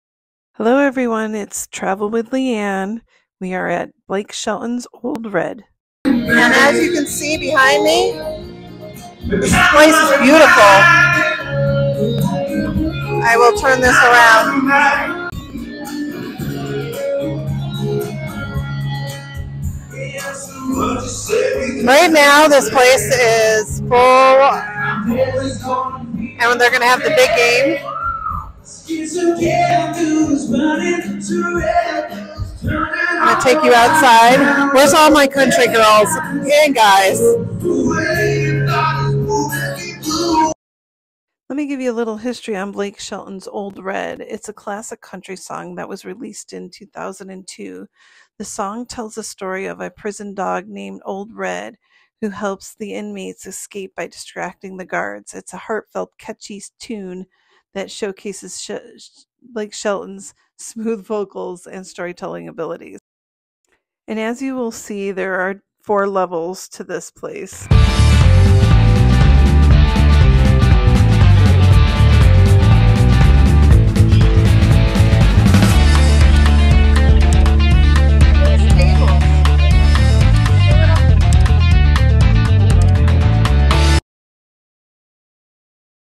Experience the Charm of Blake Shelton's Old Red in Las Vegas
“Ole Red” in Las Vegas is a country themed bar and restaurant located on the famous Las Vegas strip. It is a part of the Ole Red chain of venues, which was cofounded by country music star, Blake Shelton. The Las Vegas location offers a vibrant and lively atmosphere, featuring live music performances every day 11 AM to 1 AM. The delicious, southern inspired cuisine and a wide selection of drinks in which they have to offer, the venue, showcases Shelton’s love up for country music and provides a unique experience for fans and visitors like. So, if you’re looking for a place to enjoy some great music, mouthwatering, food, and a taste of country hospitality in Las Vegas, Ole Red is the place to be.